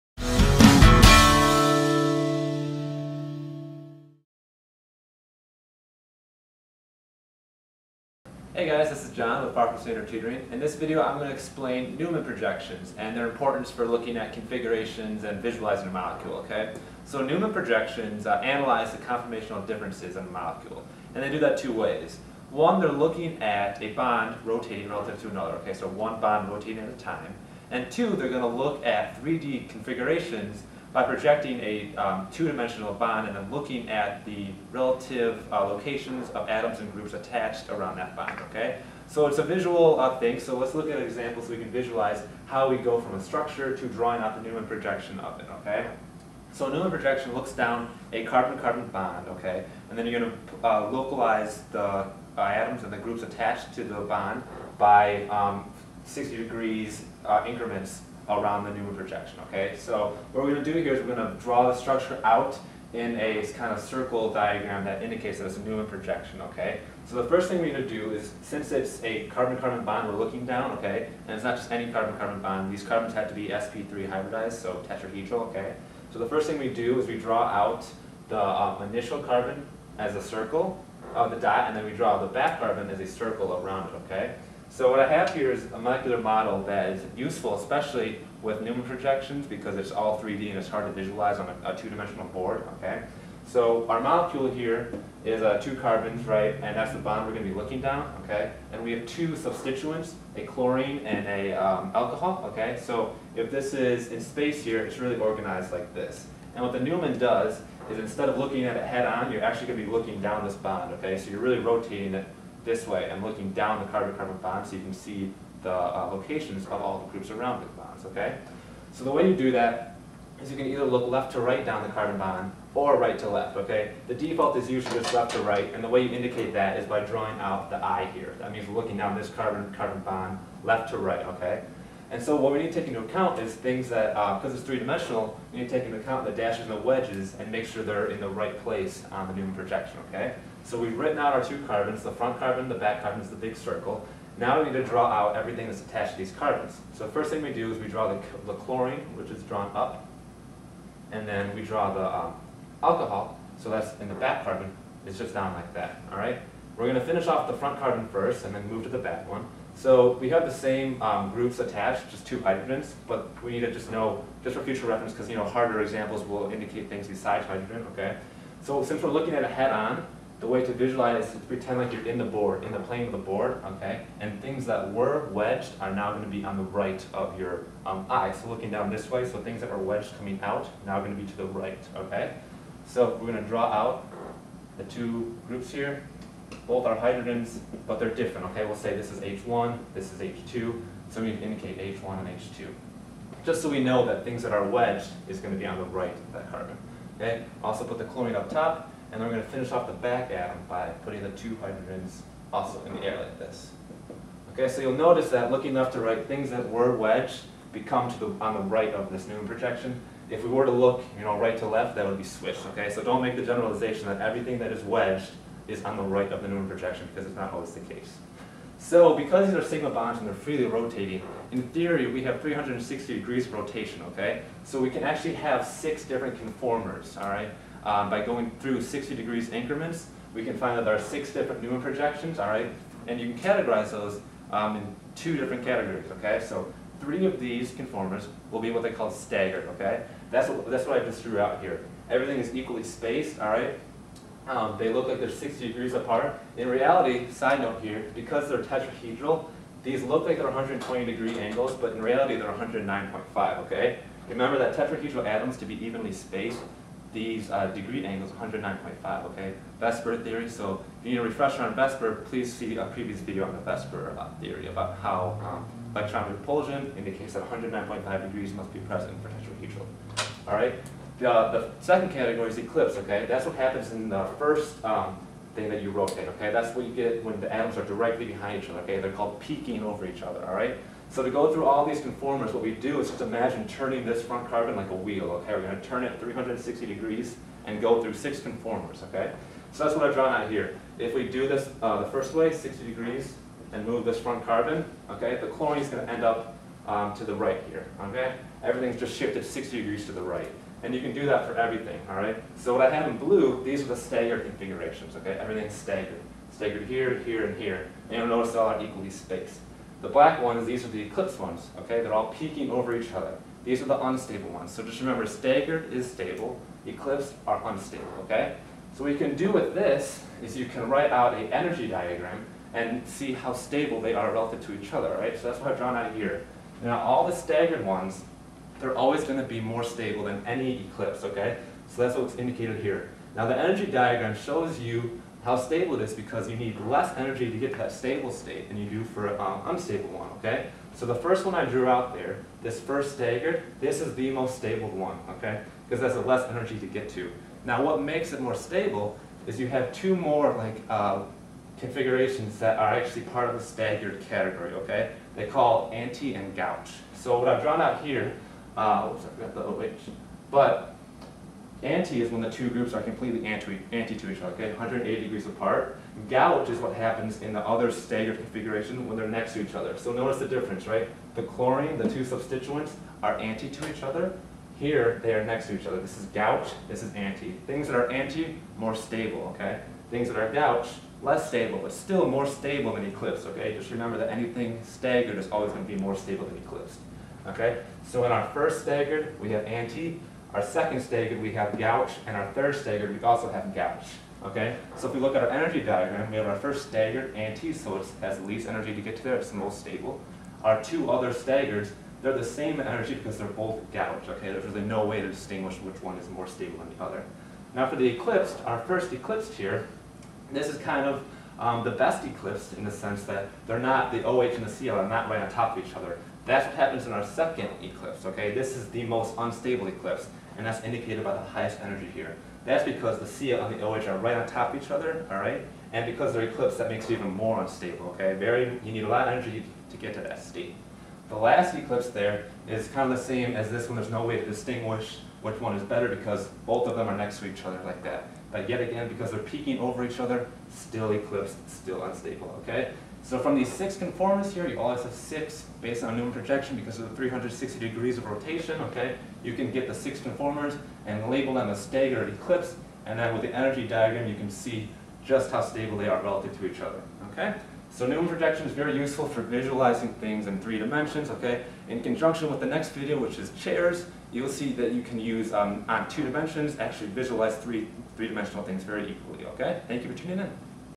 Newman Projections
John Hennessy is currently the Head of the Sciences at Far From Standard Tutoring.  He has been tutoring Organic Chemistry at the University of Michigan since 2008.  In this video, John goes over how to draw Newman projections and how they can be used to determine stability.